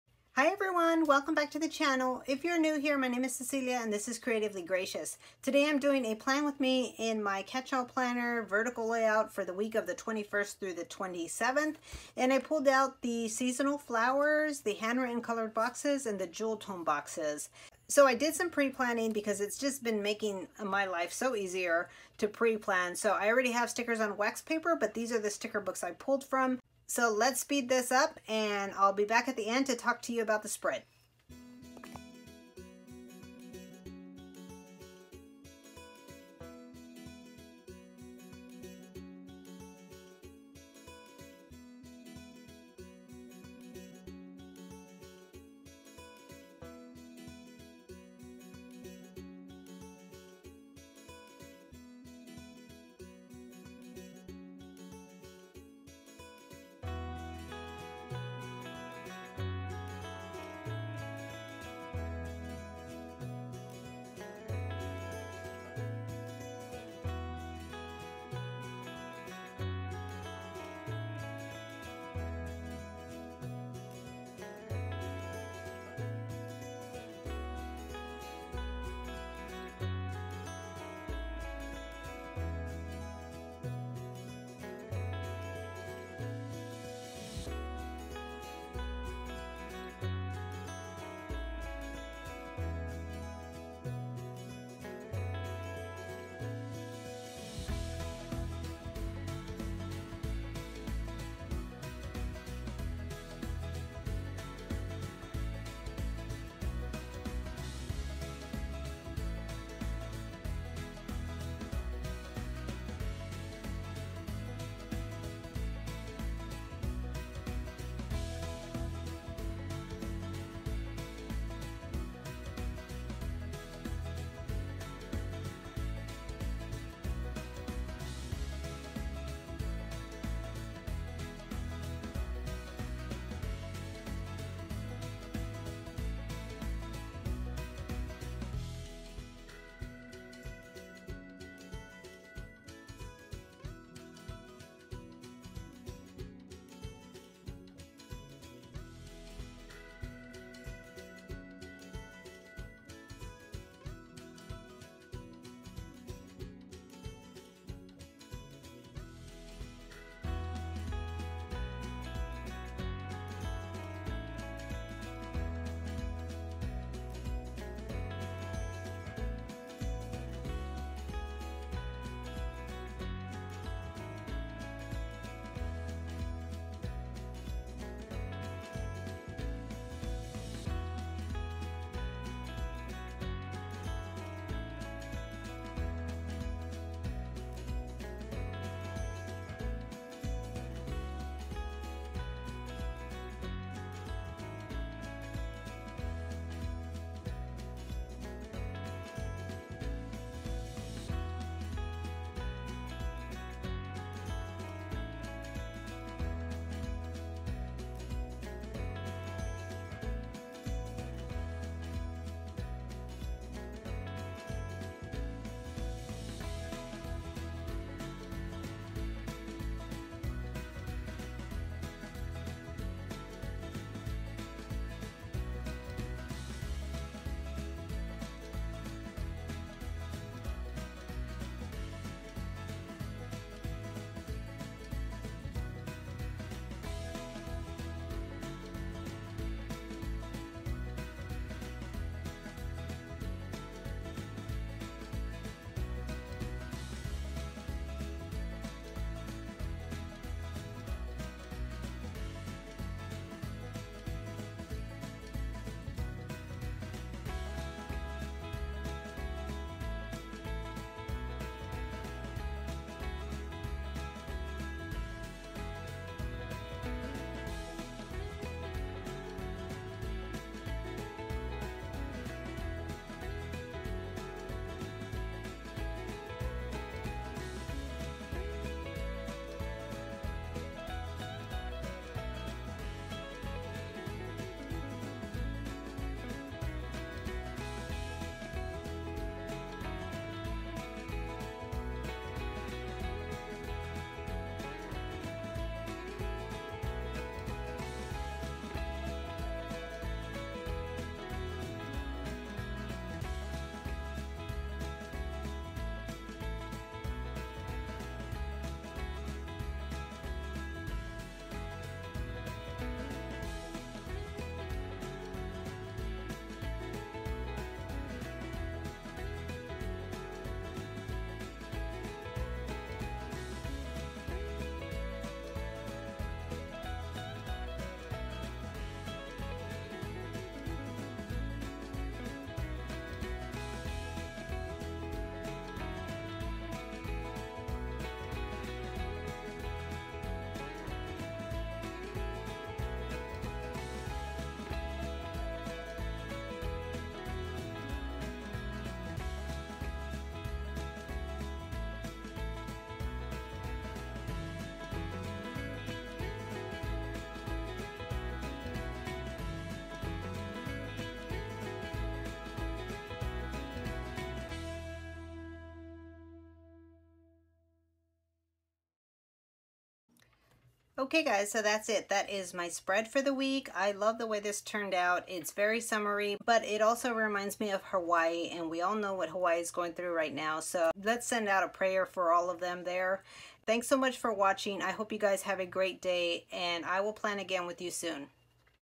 PLAN WITH ME | VERTICAL LAYOUT | HAPPY PLANNER
I know a lot of planner babes are moving on to Fall decorating but I still have the Summer vibes.